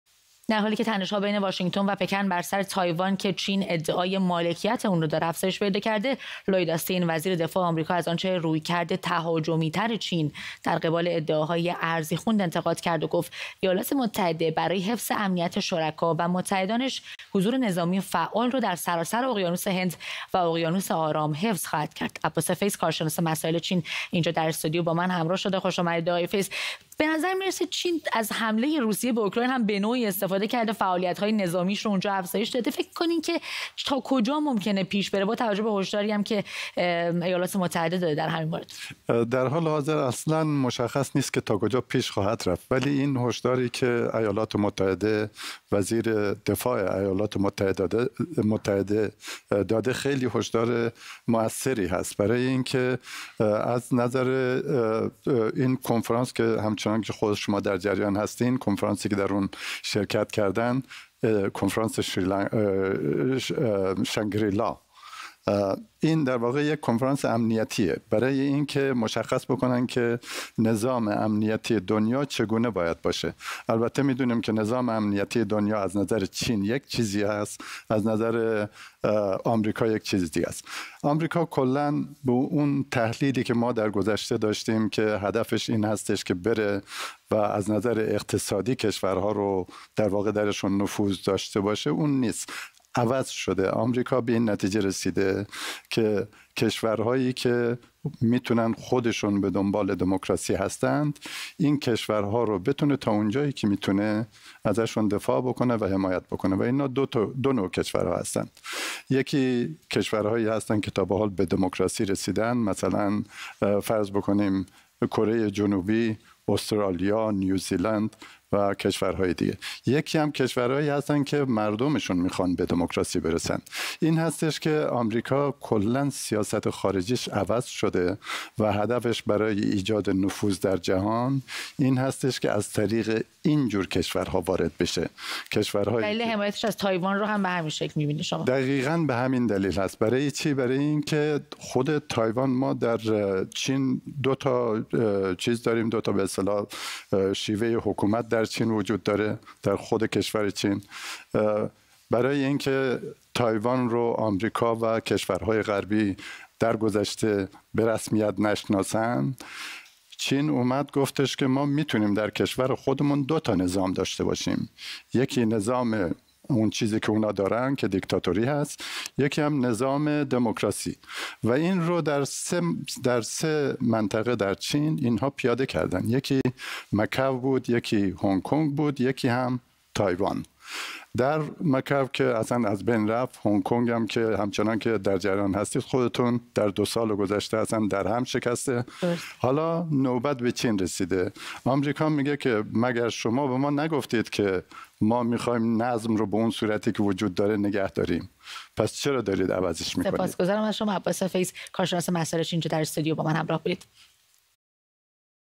دیدار وزرای دفاع آمریکا و چین همزمان با نگرانی واشینگتن از رویکرد تهاجمی‌ چین در قبال ادعاهای ارضی
گفت‌وگو با عباس فیض، کارشناس مسائل چین